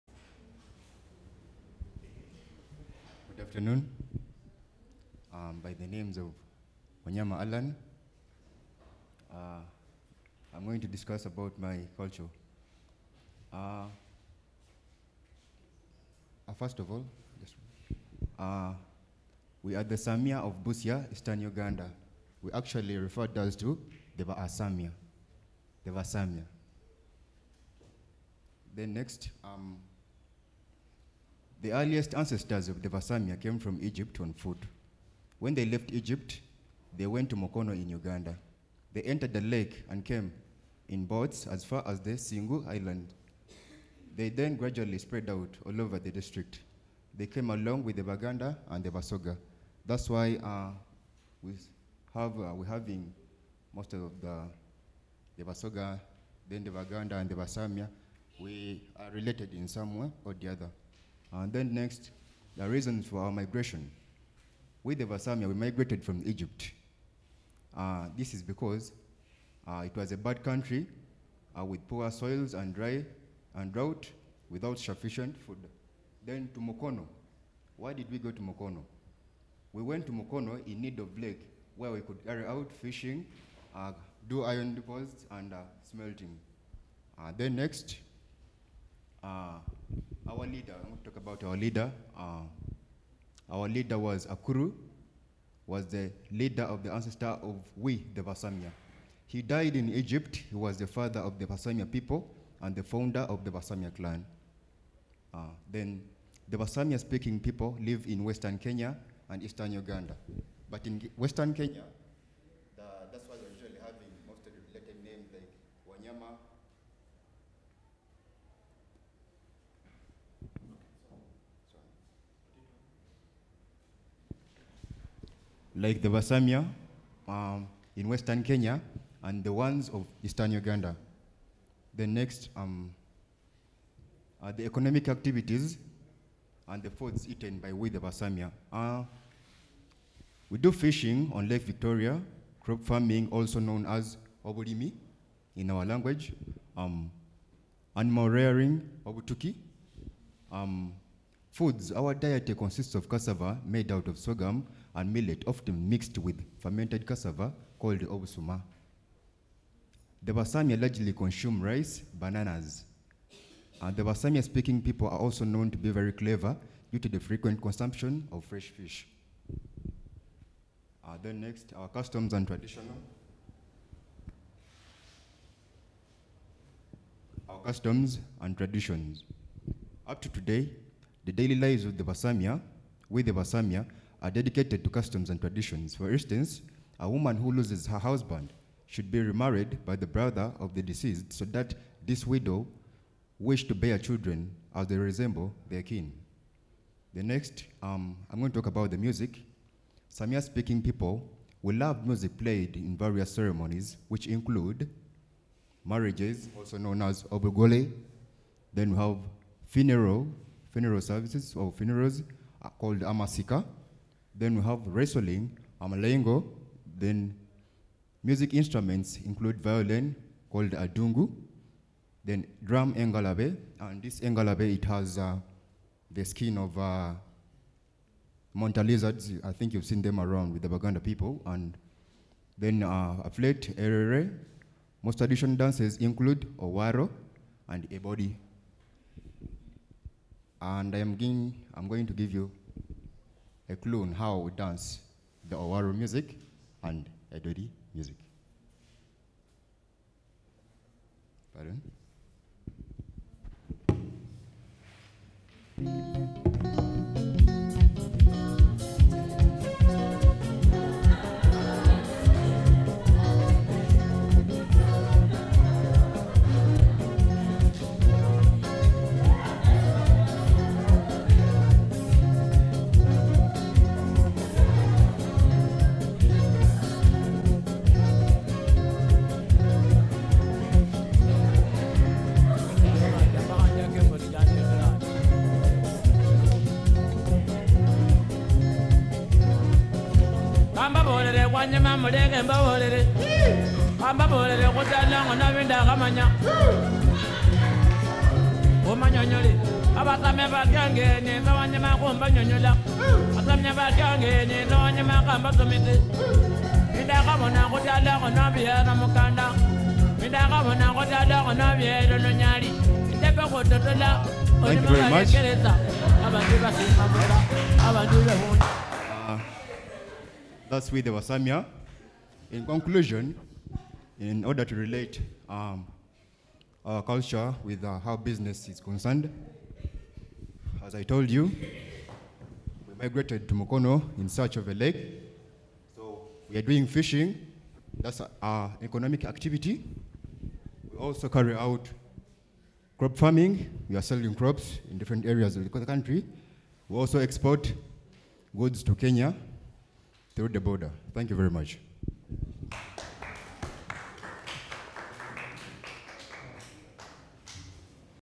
THE SAMIA OF BUSIA [EASTERN UGANDA] (Presentations on Culture & its influence on Business)
Bachelor of Entrepreneurship and Project Planning
Course Unit: Entrepreneurship
Year: 1
Lecture: Mrs Wanyenze Florence
Topic: Presentations on Culture & its influence on Business